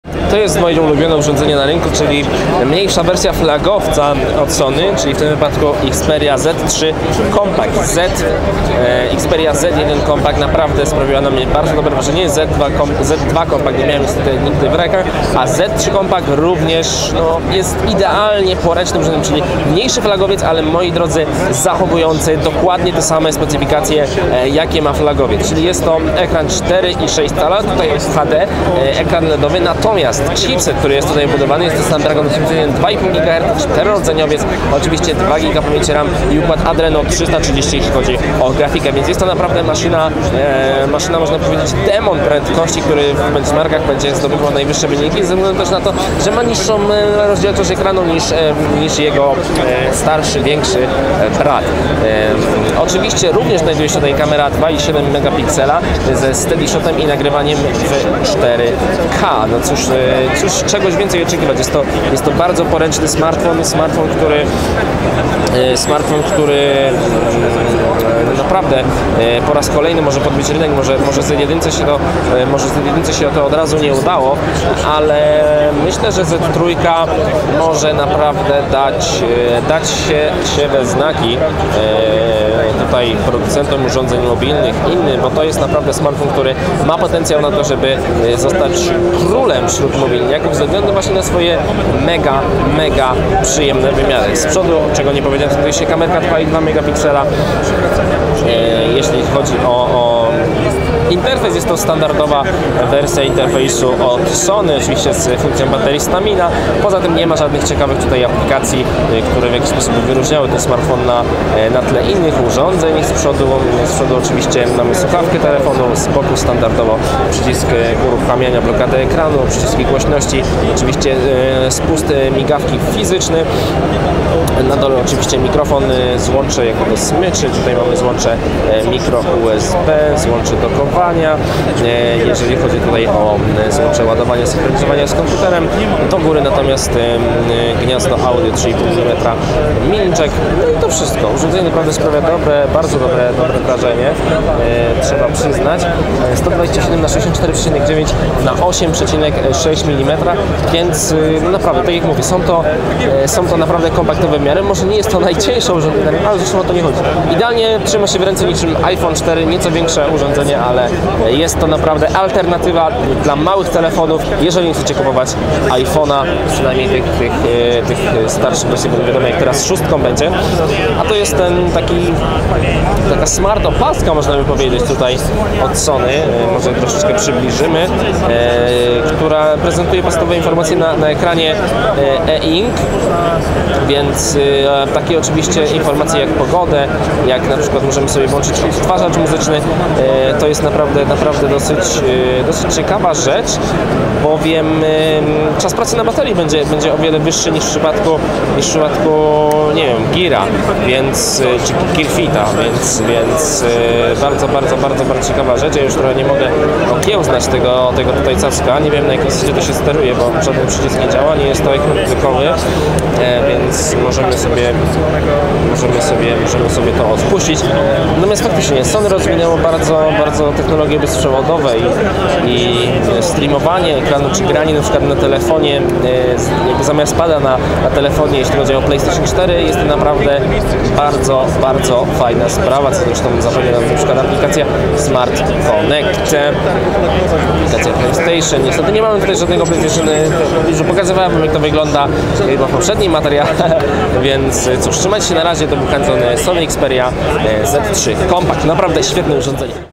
IFA 2014: Sony Xperia Z3 Compact - Najlepszy smartfon?
Sony Xperia Z3 Compact to mniejsza wersja flagowca Xperia Z3. Model ten łączy w sobie poręczność z wysoką funkcjonalnością, wydajnością oraz świetnym ekranem 4,6 cala. Szakal już miał okazję zapoznać się bliżej z tym modelem. Zobaczcie sami co z tego wynikło!

Redakcja Portalu
VideoTesty.pl